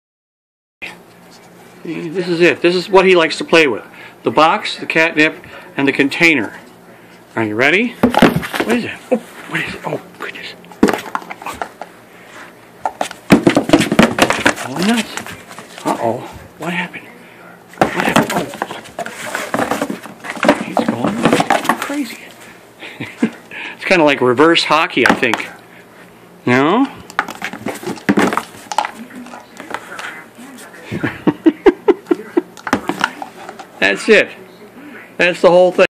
Cat hockey
he can have ANYTHING he chooses catnip container,box, with catnip, and the same handle and cloth whip he has had for YEARS. Even put the thing in the wash.